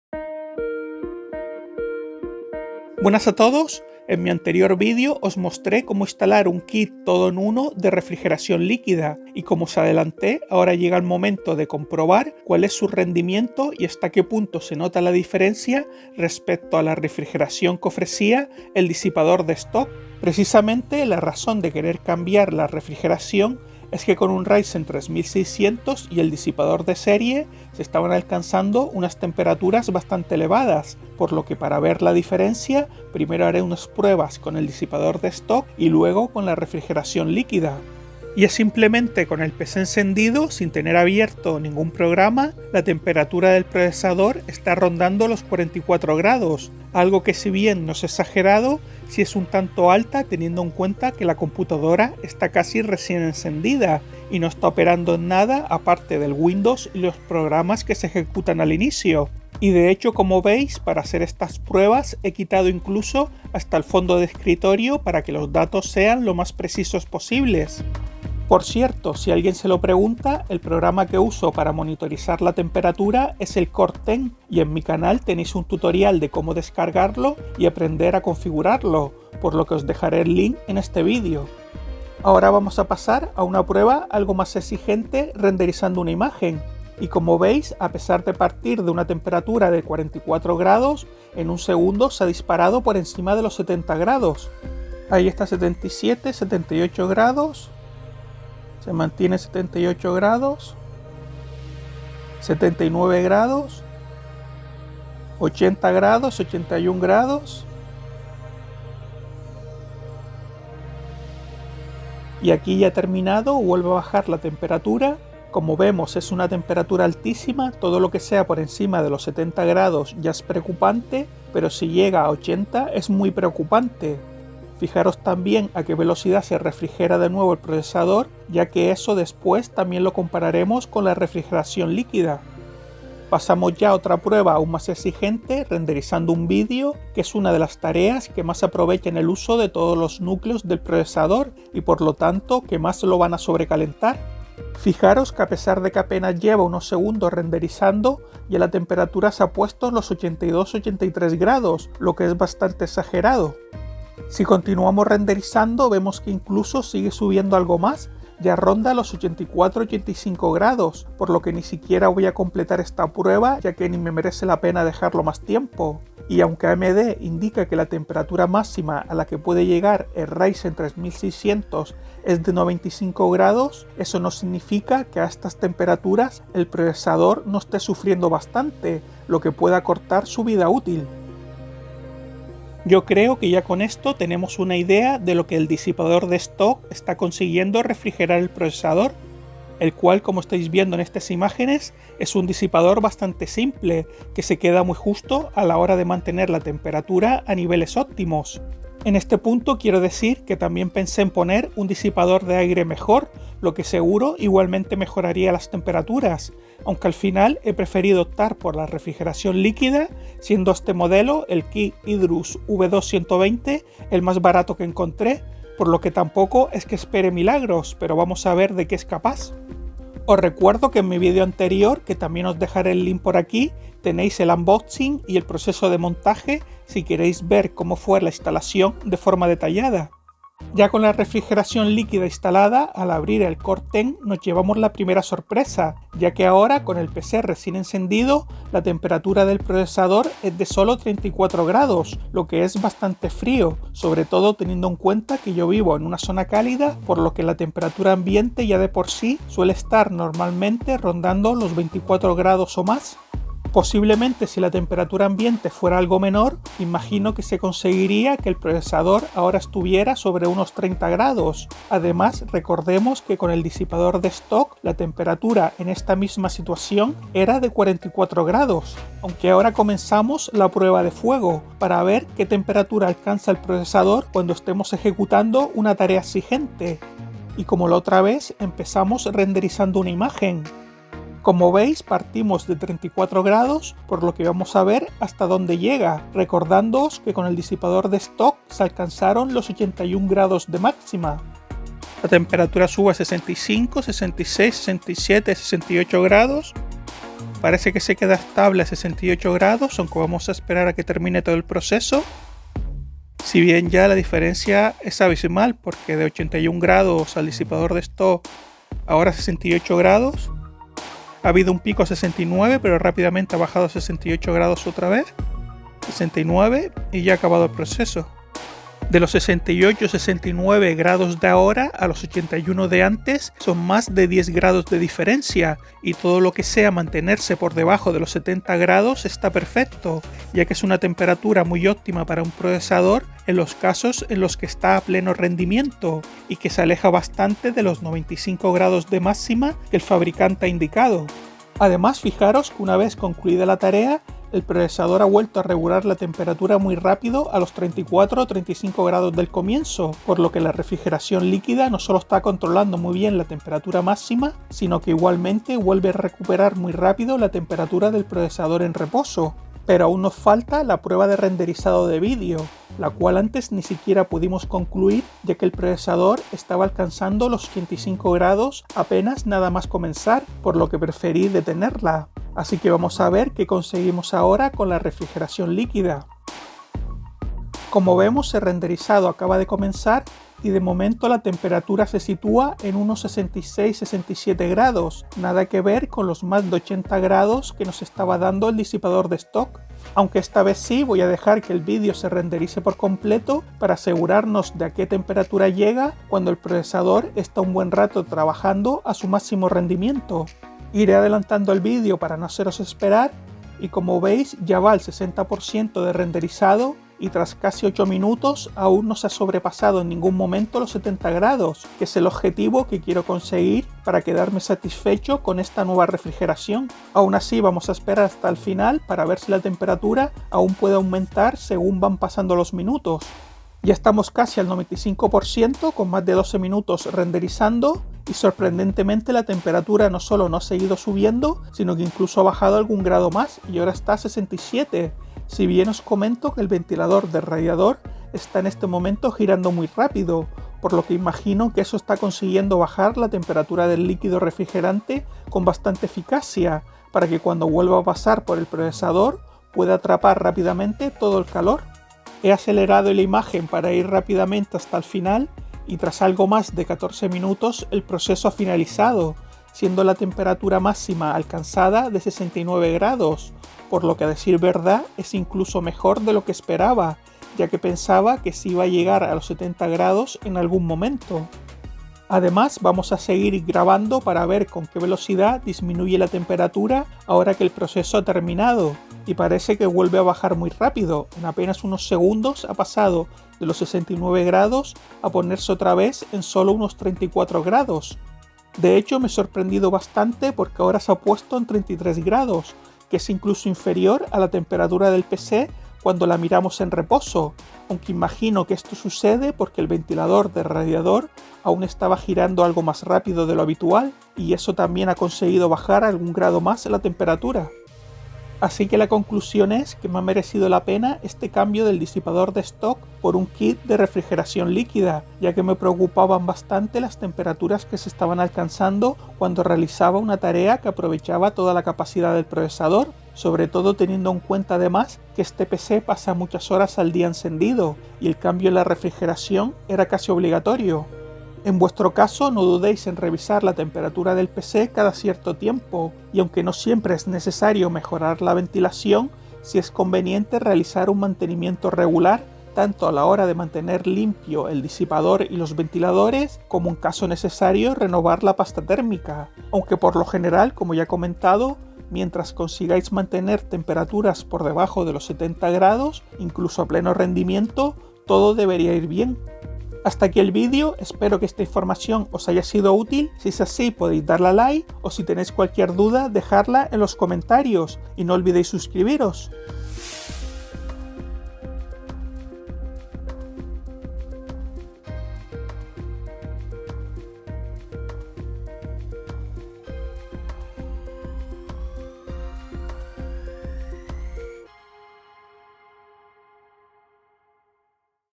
¿Se nota gran diferencia usando refrigeración liquida? | Hydrus V2 vs disipador Stock Wraith Stealth
En mí anterior vídeo os mostré como instalar un kit todo en uno de refrigeración líquida, y como os adelante ahora llega el momento de comprobar cuál es su rendimiento y hasta qué punto se nota la diferencia respecto a la refrigeración que ofrecía el disipador de Stock.

¿Se nota gran diferencia usando refrigeración liquida? | Hydrus V2 120 blue Nfortec vs disipador Stock Wraith Stealth